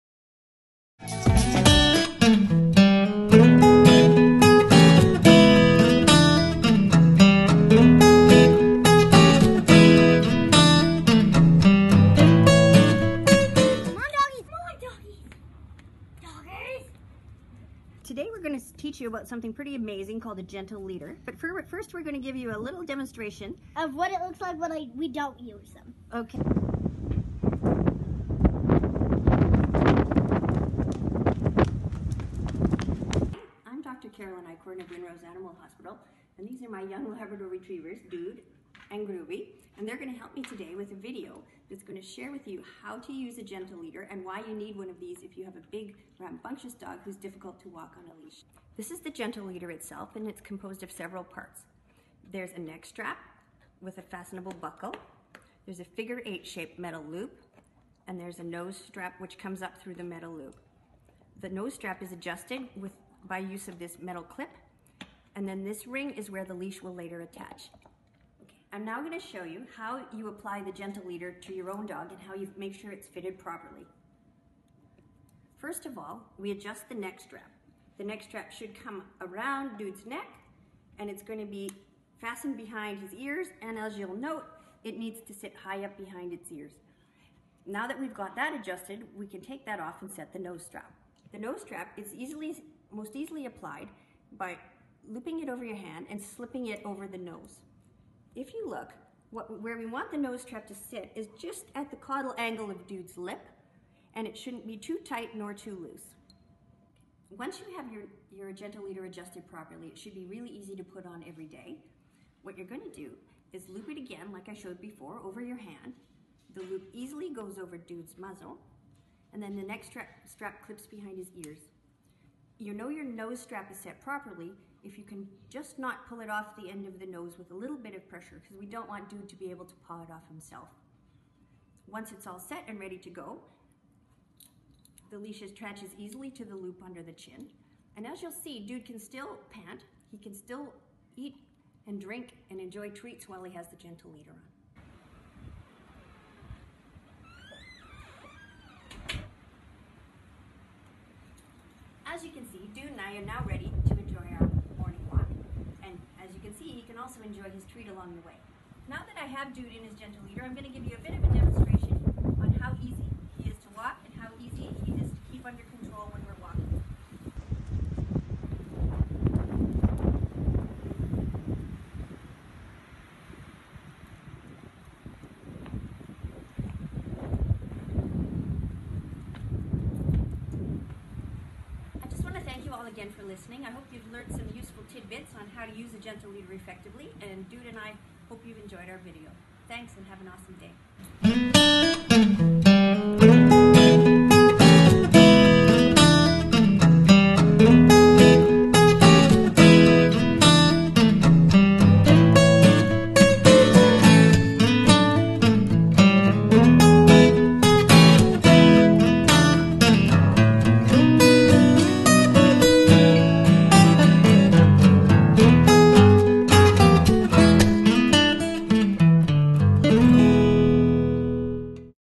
Gentle Leader 101 Small
Learn how to use a Gentle Leader for your dog